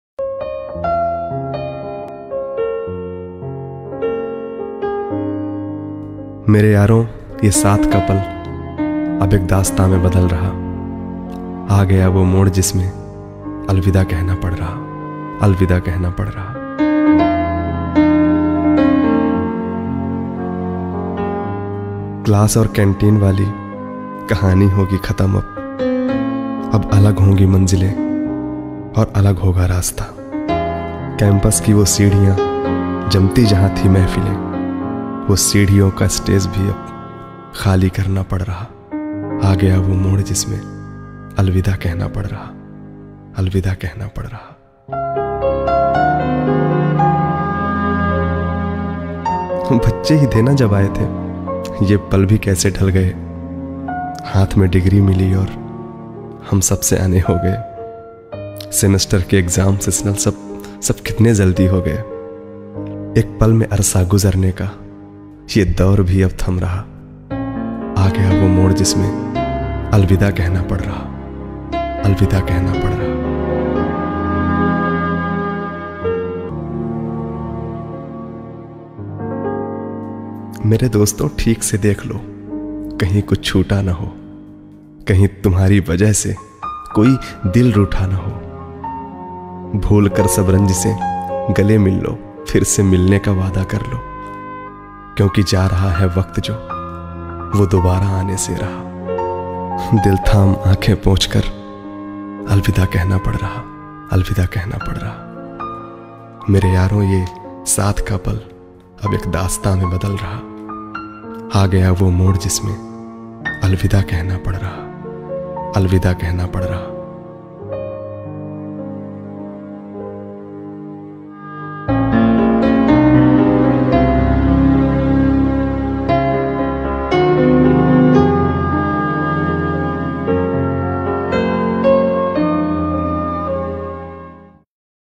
Alumni Cell | IIT Kharagpur 2022
"The story of life is quicker than the wink of an eye, the story of friendship is hello and goodbye until we meet again." True friendship is a rare commodity. Having the ability to accept each other, share their joys, carry their burdens, and offer the right advice makes for more harmonious college life. College is never easy, but your friends make it bearable and memorable. It is now time for you all to separate and head off on your separate journeys without each other. KGP and its people will always have a special place in every heart, and the good memories will never fade and that is why it doesn't matter if "आ गया वो मोड़ जिसमें..अलविदा कहना पड़ रहा"